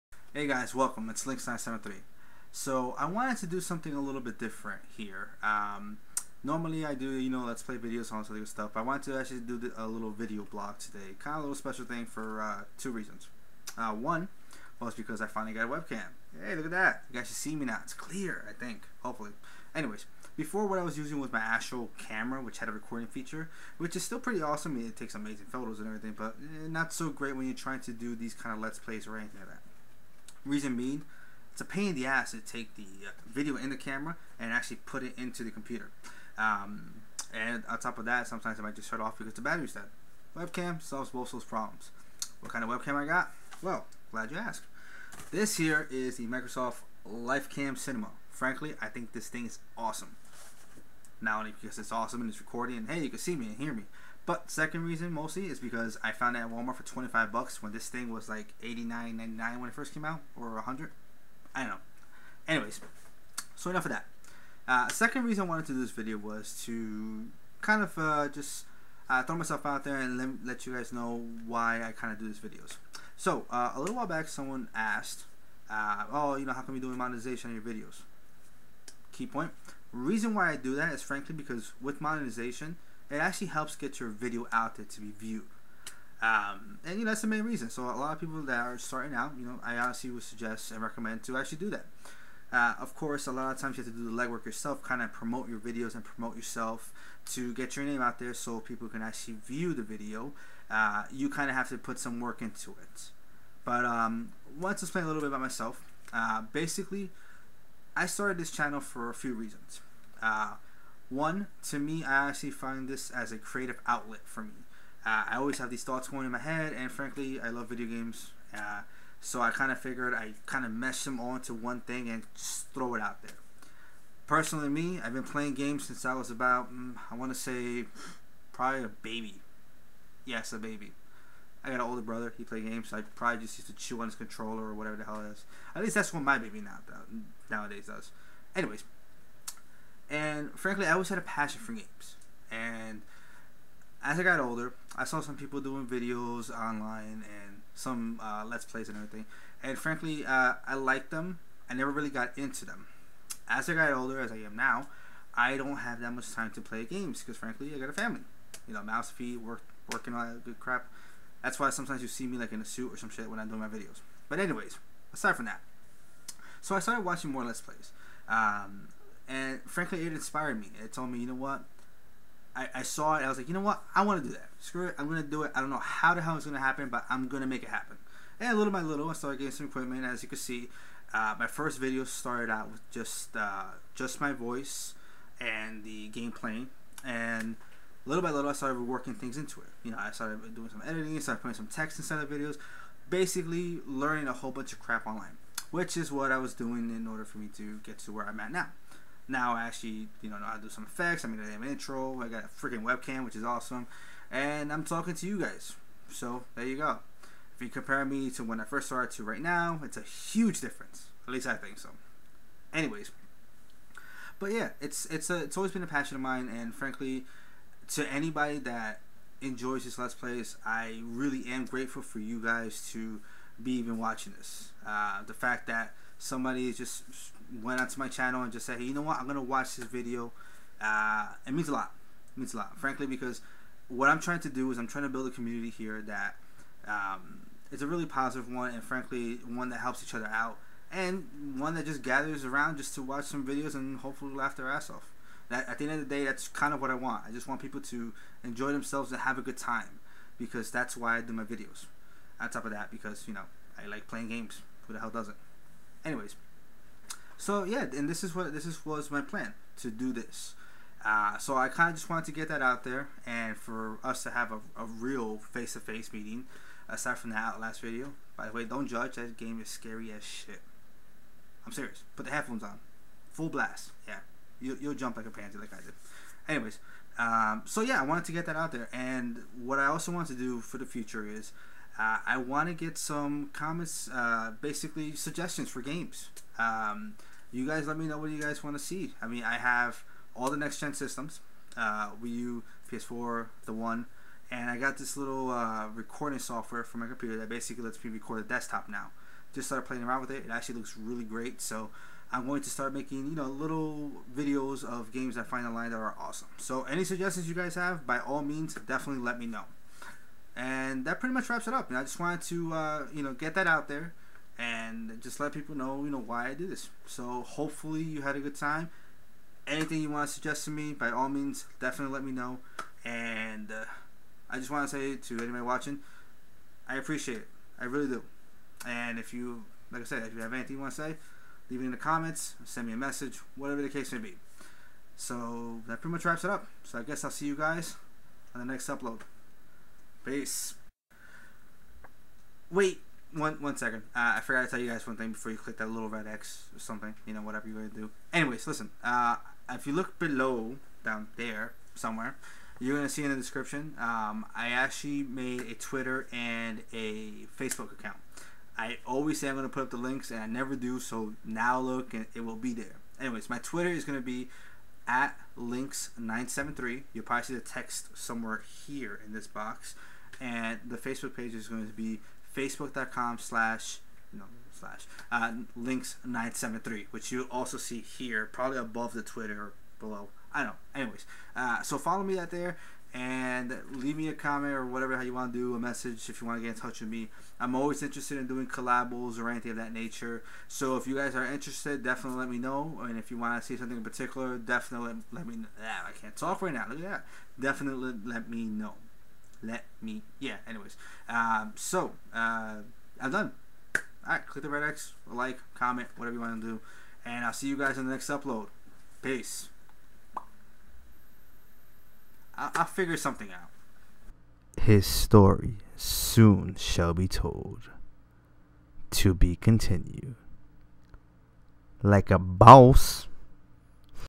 Monetization, Why I started this channel and The direction of the channel.
Before anything yes that is a watermelon the most Epic watermelon EVER.
So I made this video because I got a message and frankly I wanted to address it.
So in this video we shall discuss three topics
First : Monitization
Second: Why I started the channel
Third: The direction I would like to take the channel
Hidden ending for everyone that stayed through to the end WHAT!!! Don't be scared of his epicness
Also don't forget to give me some sudgestions of what you would like for me to play. Thank you all so much for taking the time out to view this video YOU ARE ALL AWESOME!!!